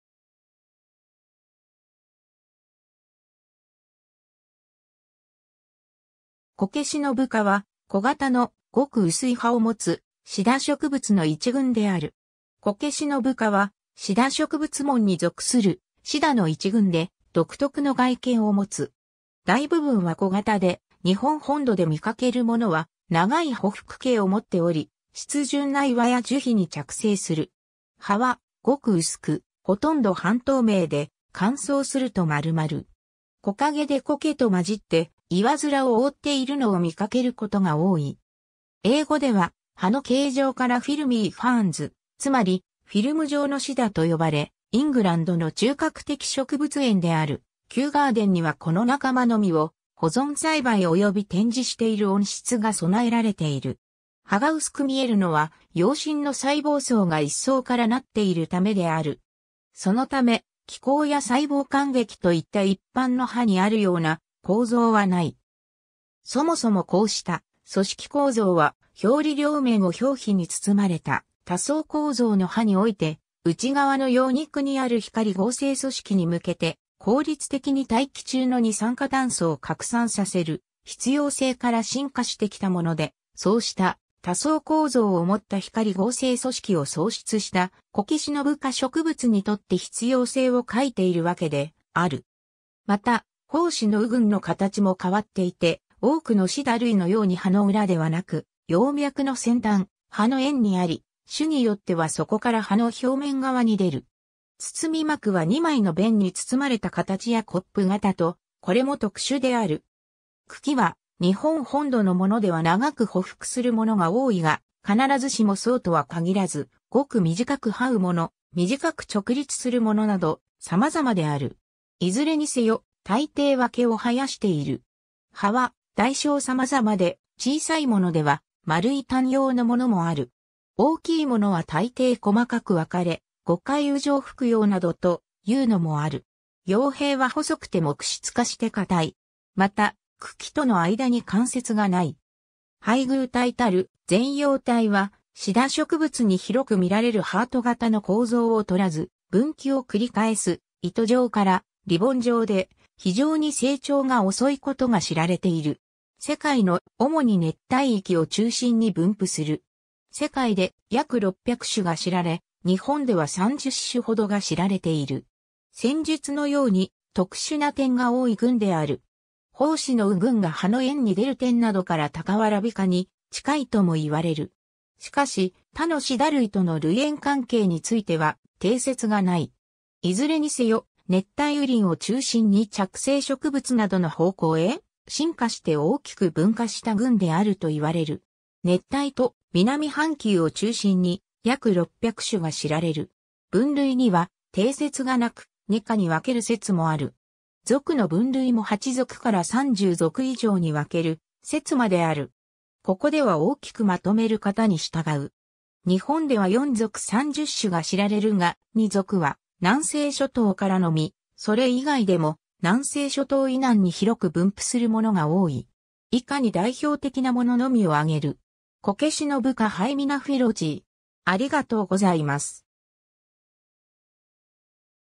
コケシノブ科
コケシノブ科 (Hymenophyllaceae) は、小型の、ごく薄い葉をもつシダ植物の一群である。
 コケシノブ科は、シダ植物門に属するシダの一群で、独特の外見をもつ。
大部分は小型で、日本本土で見かけるものは、長い匍匐茎を持っており、湿潤な岩や樹皮に着生する。
葉はごく薄く、ほとんど半透明で、乾燥すると丸まる。
木陰でコケと混じって岩面を覆っているのを見かけることが多い。
英語では葉の形状からfilmy ferns、つまり「フィルム状のシダ」と呼ばれ、イングランドの中核的植物園であるキューガーデンにはこの仲間のみを保存栽培および展示している温室が備えられている。
 葉が薄く見えるのは、葉身の細胞層が一層から成っているためである。
そのため、気孔や細胞間隙と言った一般の葉にあるような構造はない。
そもそもこうした組織構造は、表裏両面を表皮に包まれた多層構造の葉に於いて、内側の葉肉にある光合成組織に向けて効率的に大気中の二酸化炭素を拡散させる必要性から進化してきたもので、そうした多層構造をもった光合成組織を喪失したコケシノブ科植物にとって必要性を欠いているわけである。
また、胞子のう群の形も変わっていて、多くのシダ類のように葉の裏ではなく、葉脈の先端、葉の縁にあり、種によってはそこから葉の表面側に出る。
包膜は二枚の弁に包まれた形（二弁状）やコップ型と、これも特殊である。
 茎は、日本本土のものでは長く匍匐するものが多いが、必ずしもそうとは限らず、ごく短く這うもの、短く直立するものなど、さまざまである。
いずれにせよ、たいていは毛を生やしている。
葉は大小様々で、小さいものでは丸い単葉のものもある。
大きいものはたいてい細かく分かれ、五回羽状複葉などというのもある。
葉柄は細くて木質化して硬い。
また、茎との間に関節がない。
 配偶体たる前葉体はシダ植物に広くみられるハート形の構造をとらず、分岐を繰り返す糸状からリボン状で、非常に成長が遅いことが知られている。
 世界の、主に熱帯域を中心に分布する。
世界で約600種が知られ、日本では30種ほどが知られている。
 先述のように、特殊な点が多い群である。
胞子のう群が葉の縁に出る点などからタカワラビ科に近いとも言われる。
しかし、他のシダ類との類縁関係については定説がない。
いずれにせよ、熱帯雨林を中心に着生植物などの方向へ進化して大きく分化した群であると言われる。
 熱帯と南半球を中心に約600種が知られる。
分類には定説がなく、二科に分ける説もある。
属の分類も8属から30属以上に分ける説まである。
ここでは大きくまとめる方に従う。
 日本では4属30種が知られるが、2属は南西諸島からのみ、それ以外でも南西諸島以南に広く分布するものが多い。
以下に代表的なもののみを挙げる。
 コケシノブ科 Hymenophyllaceae